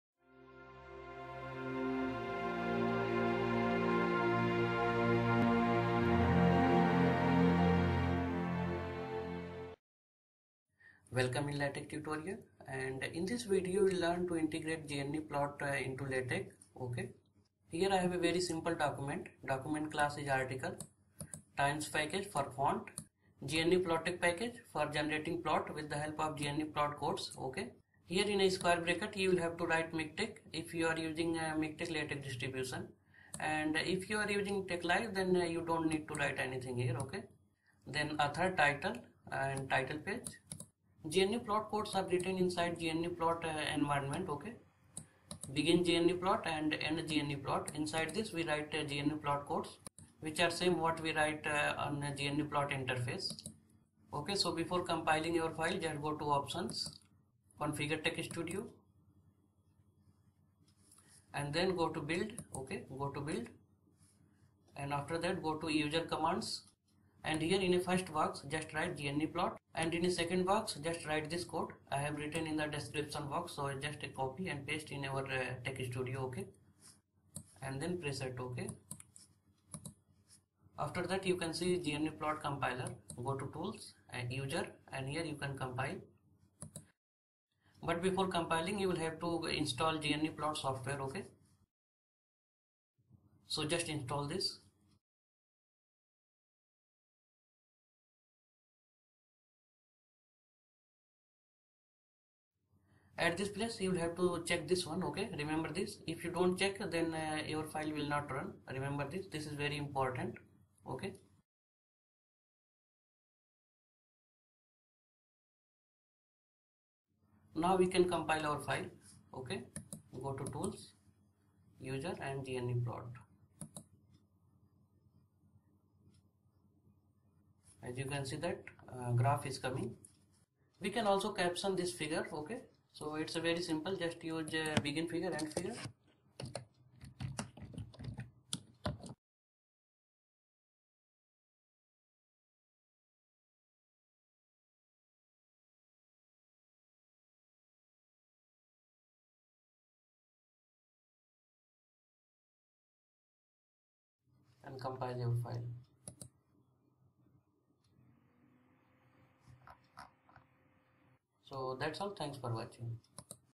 How to Use Gnuplot into LaTeX (LaTeX: Tips/Solution-16)
In this video, I will demonstrate the easiest way to use gnuplot into TeXstudio.